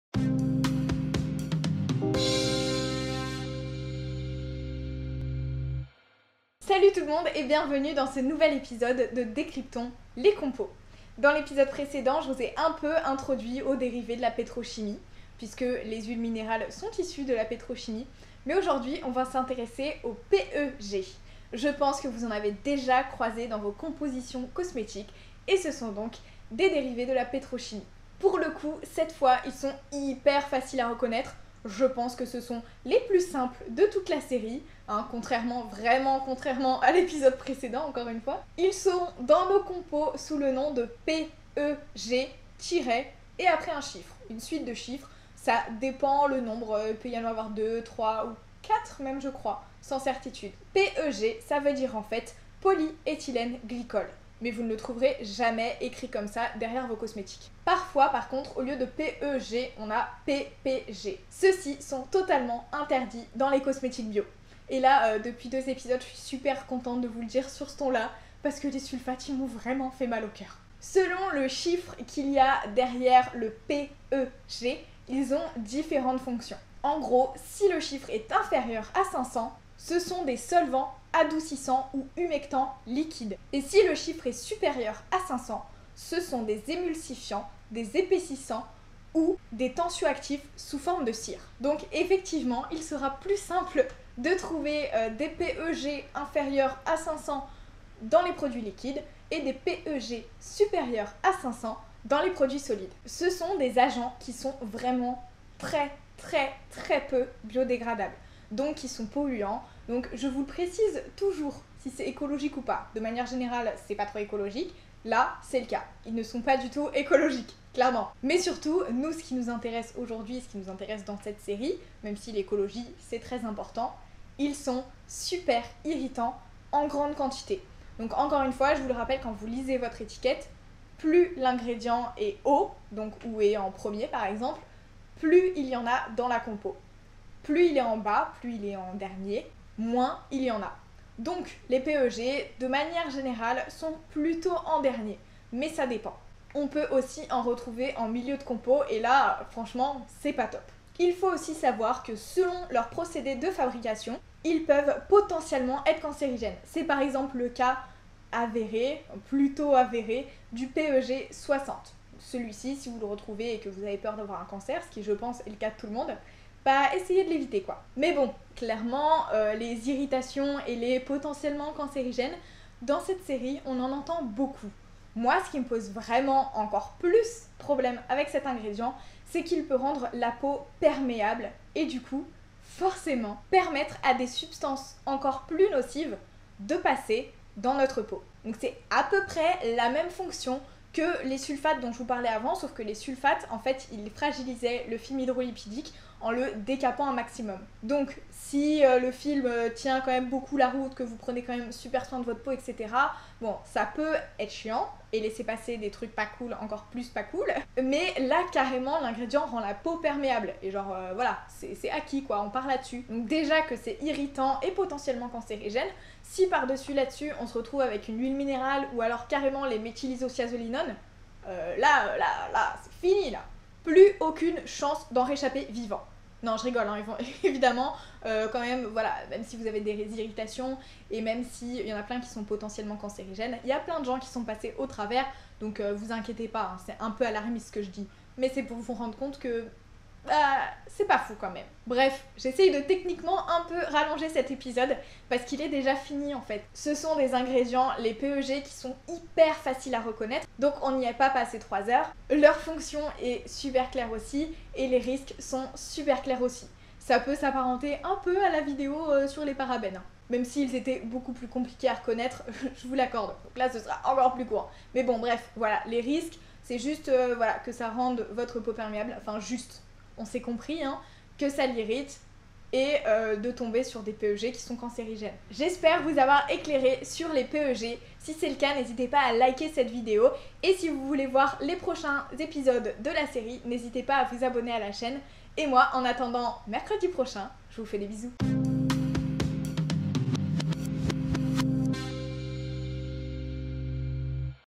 Les PEG | Décryptons les compos #6
Dans ce cinquième numéro, on essaie de reconnaître toutes les formes d'huiles minérales, des composants totalement occlusifs !

La suite à venir...